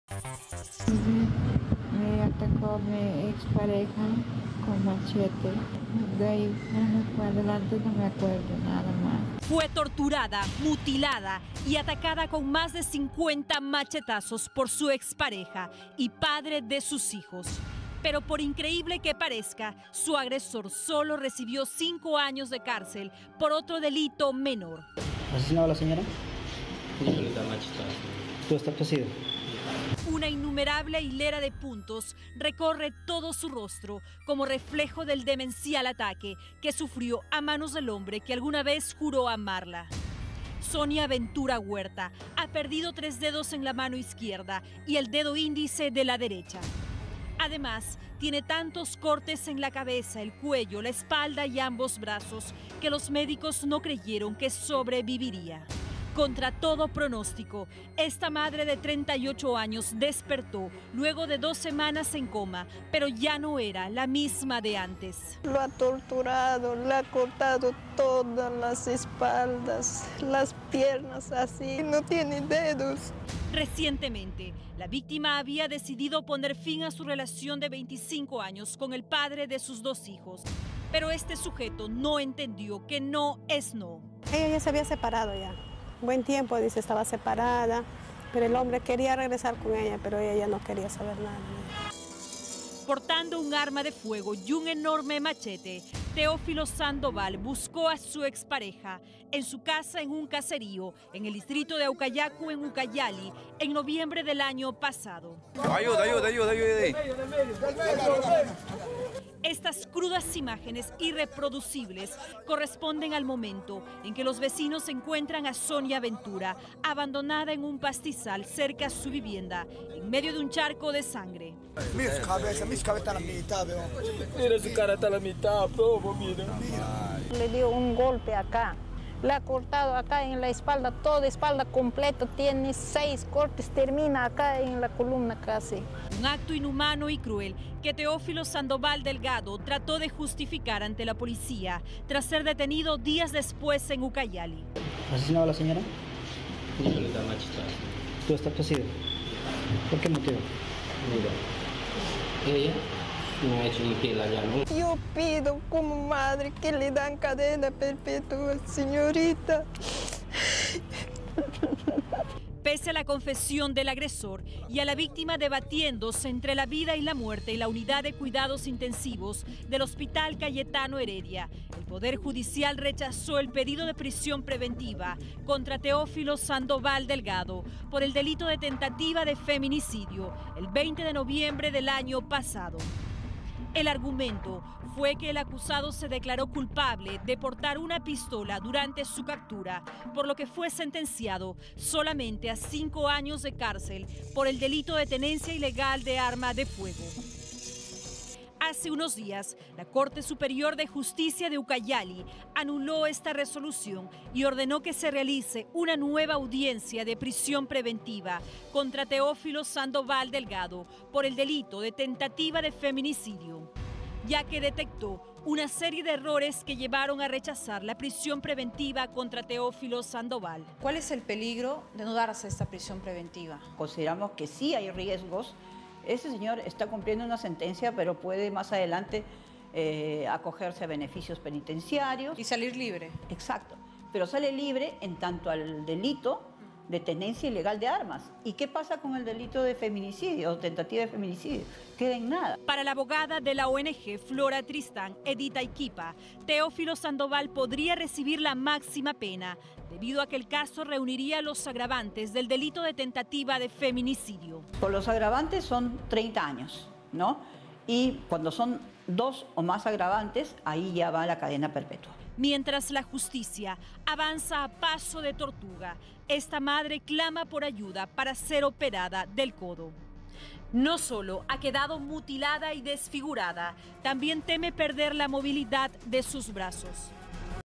Mujer pide justicia tras sobrevivir a machetazos de expareja que recibió 5 años de cárcel
Teófilo Sandoval fue el desalmado sujeto que la atacó sin ningún reparo luego que ella quisiera romper la relación.